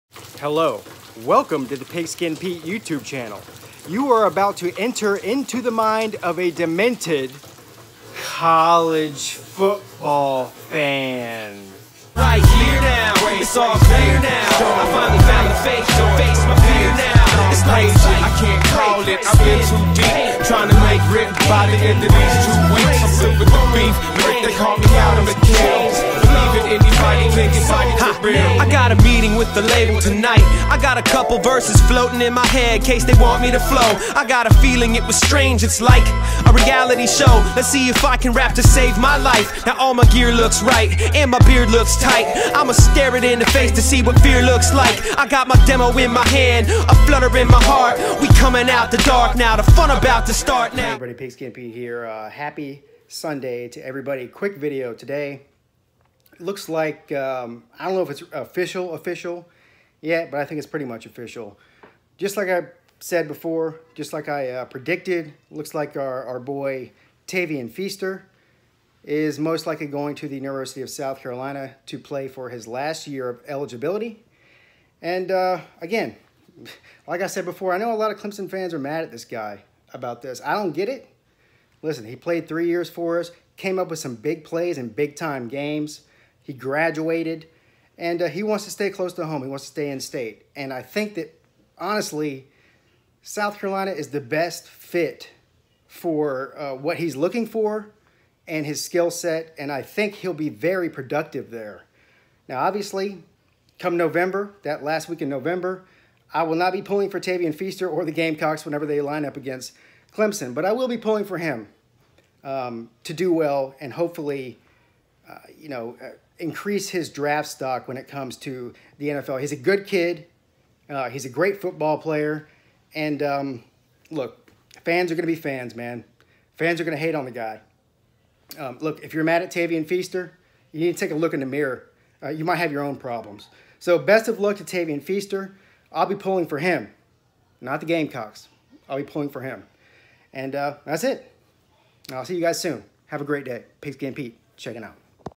Clemson RB Tavien Feaster Likely To Transfer To South Carolina | Reports Say
Clemson RB Tavien Feaster likely will announce his transfer to University of South Carolina for his final season!